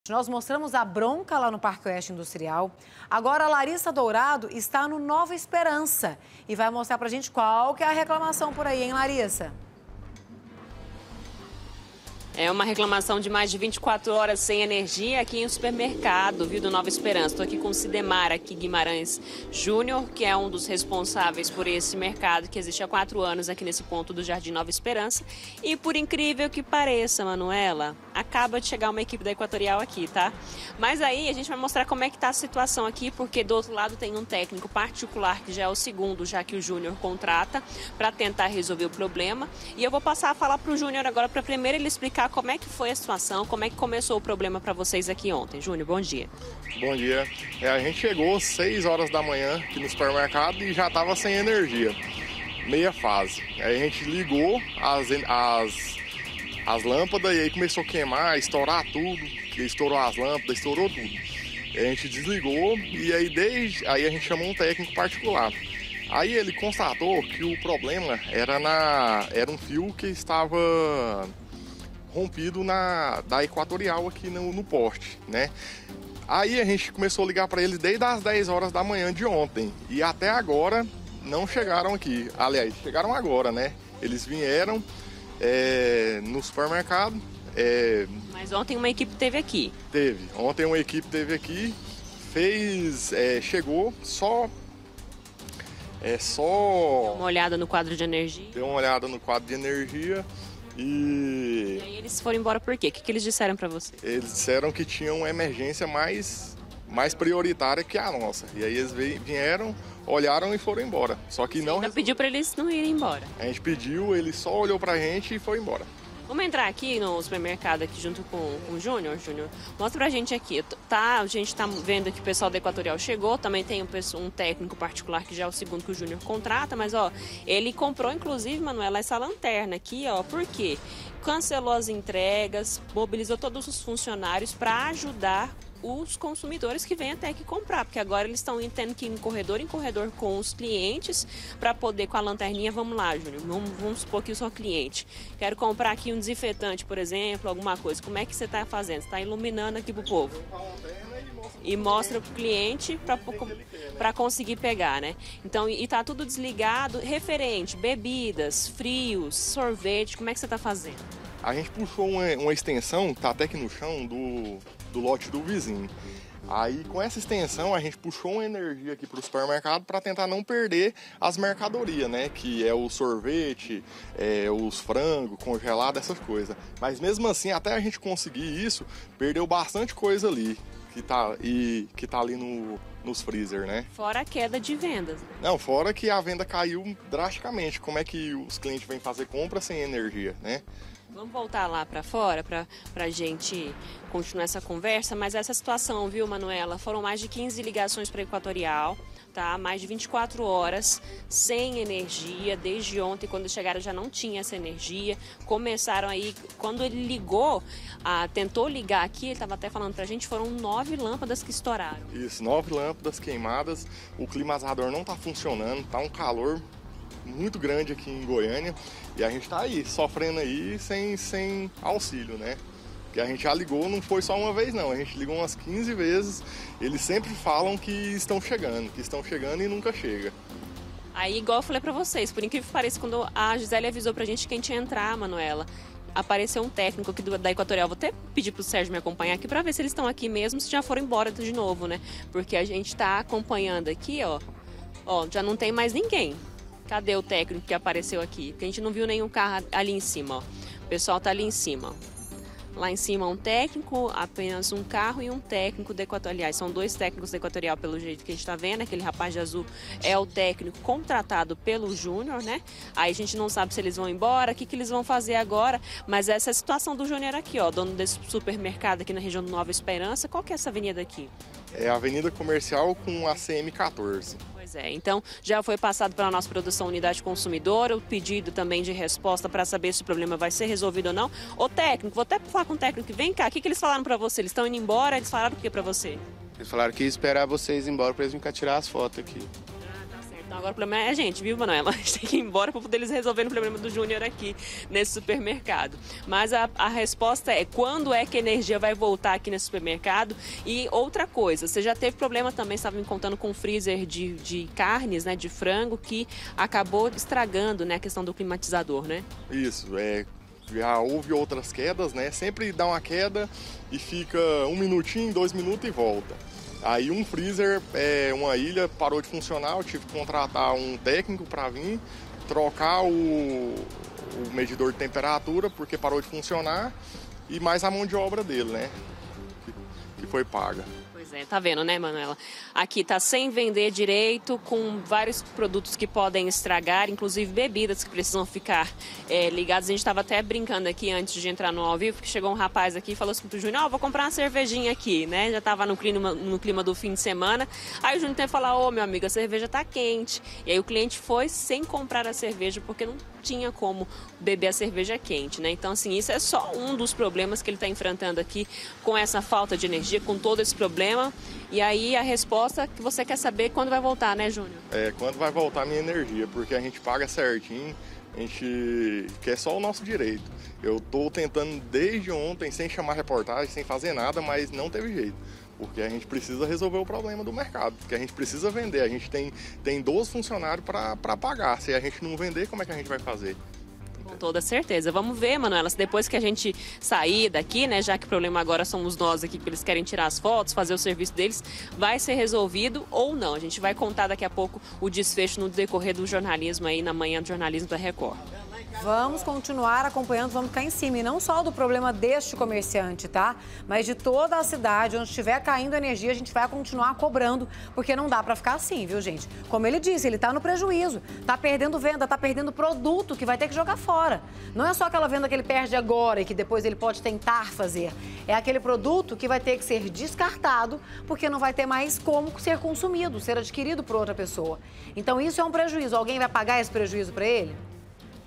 FALTA DE ENERGIA: E AS RECLAMAÇÕES DA FALTA DE ENERGIA NÃO PARAM.
MOSTRAMOS A BRONCA LÁ NO PARQUE OESTE INDUSTRIAL E AGORA A LARISSA DOURADO ESTÁ NO NOVA ESPERANÇA. LARISSA, QUAL É A RECLAMAÇÃO POR AÍ?